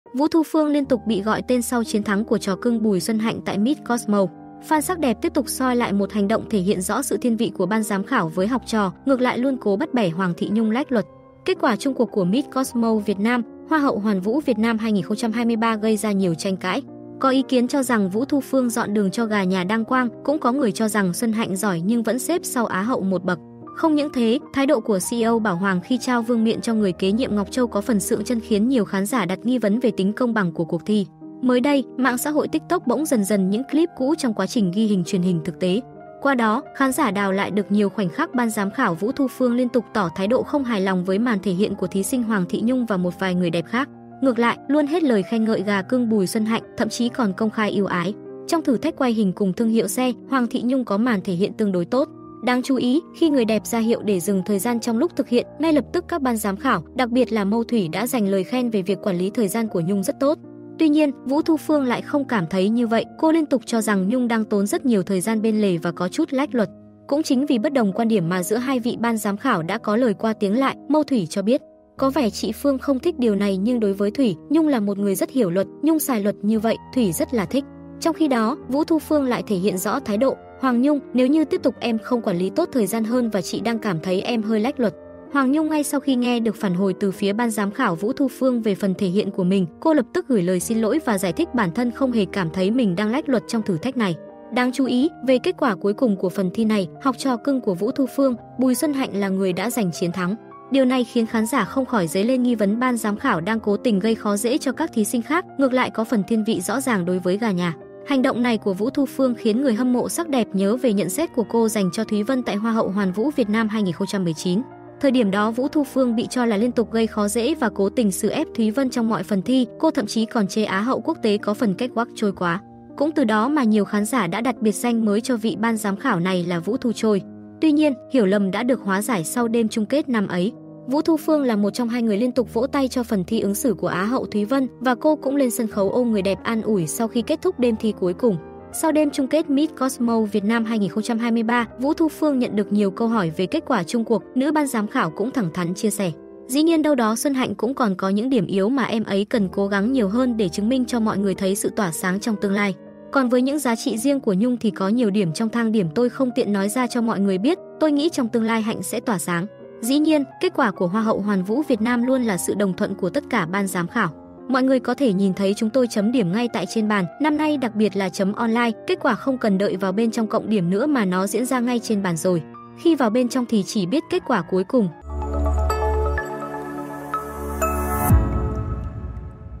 Vũ Thu Phương bị "đào" khoảnh khắc bắt bẻ Á hậu
Vũ Thu Phương liên tục bị gọi tên sau chiến thắng của trò cưng Bùi Xuân Hạnh tại Miss Cosmo. Fan sắc đẹp tiếp tục soi lại một hành động thể hiện rõ sự thiên vị của BGK với học trò, ngược lại luôn cố bắt bẻ Hoàng Thị Nhung lách luật.

Kết quả chung cuộc của Miss Cosmo Vietnam - Hoa hậu Hoàn vũ Việt Nam 2023 gây ra nhiều tranh cãi. Có ý kiến cho rằng Vũ Thu Phương “dọn đường” cho gà nhà đăng quang, cũng có người cho rằng Xuân Hạnh giỏi nhưng vẫn xếp sau Á hậu một bậc. Không những thế, thái độ của CEO Bảo Hoàng khi trao vương miện cho người kế nhiệm Ngọc Châu có phần “sượng trân” khiến nhiều khán giả đặt nghi vấn về tính công bằng của cuộc thi.